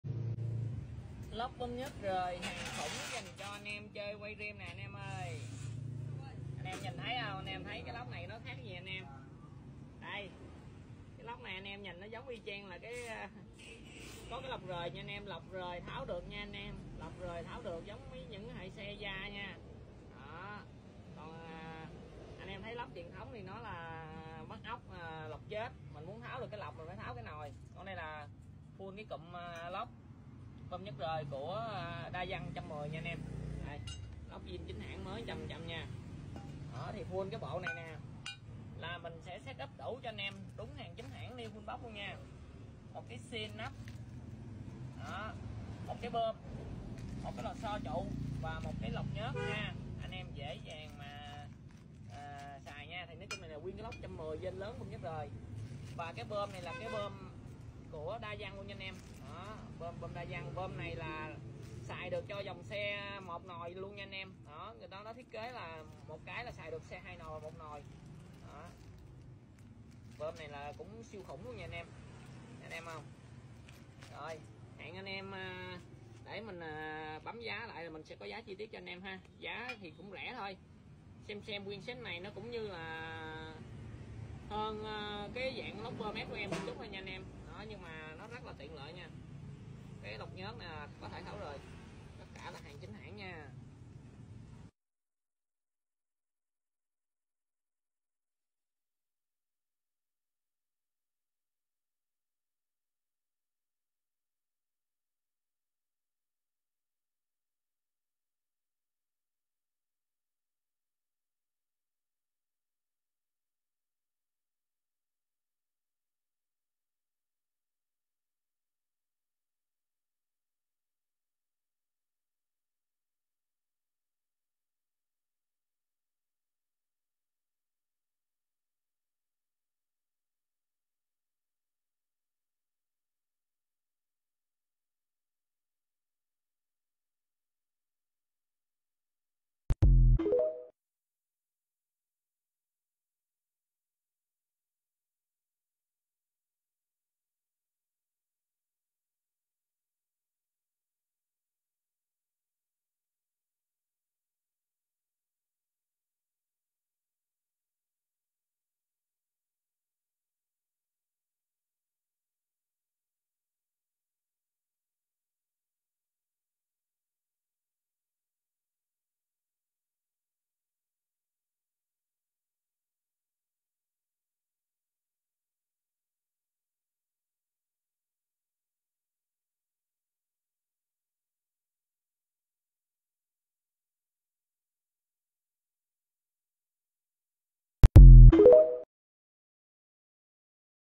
🇻🇳 950k 🌽 BỘ LÓC BƠM NHỚT RỜI 110 DAYANG ZIN CÓ LỌC NHỚT RỜI GIỐNG YA CHÍNH HÃNG DY
🌽  BỘ LÓC BƠM NHỚT RỜI 110 DAYANG ZIN CÓ LỌC NHỚT RỜI GIỐNG YA CHÍNH HÃNG DY CHO HỆ 2 NỒI DREAM WAVE VÀ CUB 67 CD CL 1 NỒI

🔥 Giá: 950K  / BỘ LỐC 110 + BƠM 110 + PHỤ KIỆN

☘️ Chất lượng 9/10 và giá chỉ 1/3 so với Zin 110 Thái theo xe

☘️  sử dụng ok để làm vào máy đôn - máy độ - đắp lốc xổ trái

              Alo Mobile - Zalo - Facebook :
          ☎️0️⃣9️⃣3️⃣3️⃣.9️⃣9️⃣6️⃣.3️⃣3️⃣2️⃣

👉 168 đường Thới Hòa, Xã Vĩnh Lộc A, Huyện Bình Chánh, TPHCM - Ngay trong đường số 7 Khu Công Nghiệp Vĩnh Lộc